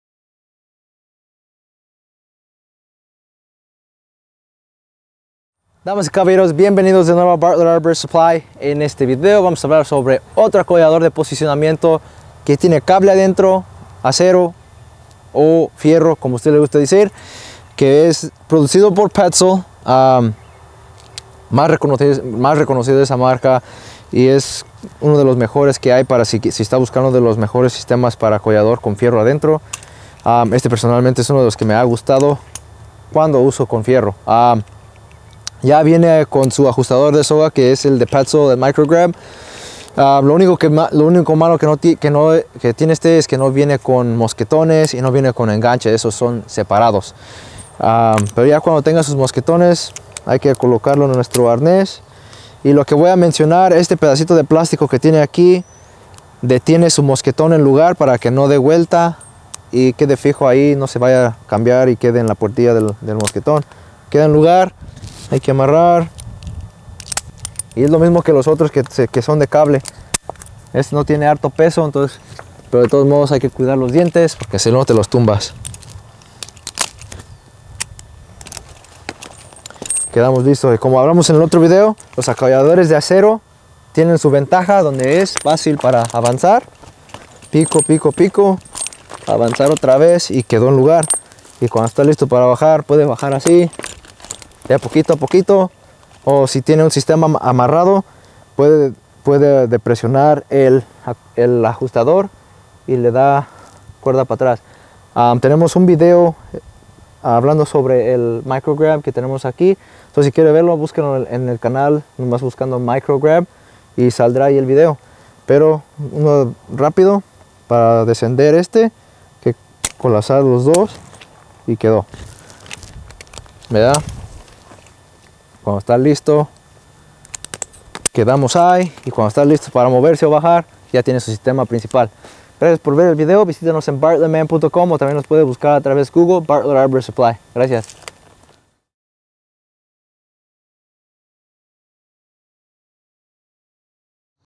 El acollador perfecto de acero!
El acollador Microflip fabricado por Petzl, es uno de los mejores que se pueda encontrar hoy en día.